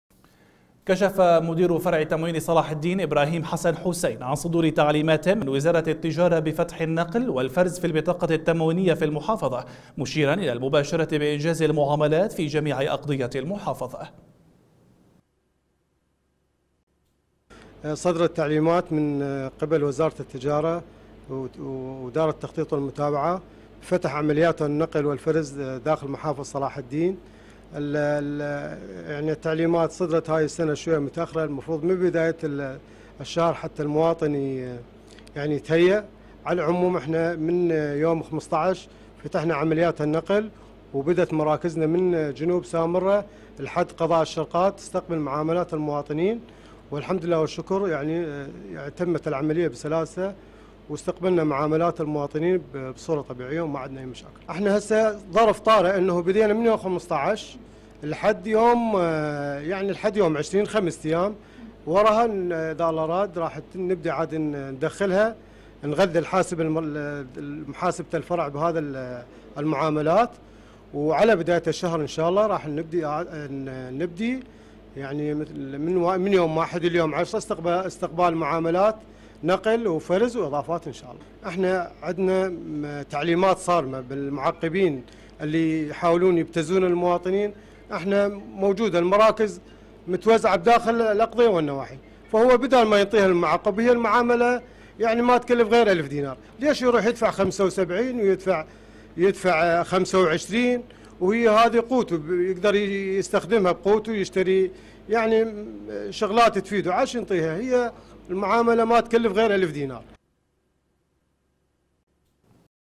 ابراهيم حسن حسين – مدير فرع تموين المحافظة
كشف مدير فرع تموين صلاح الدين ابراهيم حسن حسين عن صدور تعليمات من وزارة التجارة بفتح النقل والفرز في البطاقة التموينية في المحافظة , مشيرا الى المباشرة بإنجاز المعاملات في جميع اقضية المحافظة